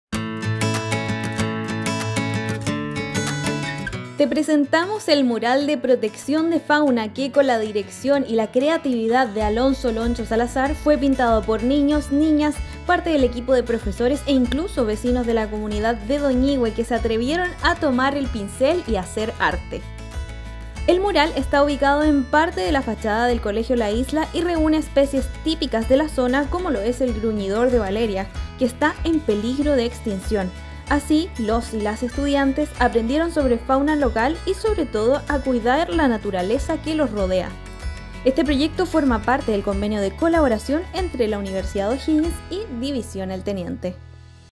La comunidad de Doñihue estrenó su nuevo mural de protección de fauna en el Colegio La Isla
El mural de protección de fauna es una iniciativa que se realizó en conjunto con la Municipalidad de Doñihue, la Universidad de O'Higgins y DivisiónElTeniente 👏👏 Esta obra se ubica en la fachada del Colegio La Isla y fue pintada por estudiantes, apoderados, profesores, vecinos y vecinas 🫂✨️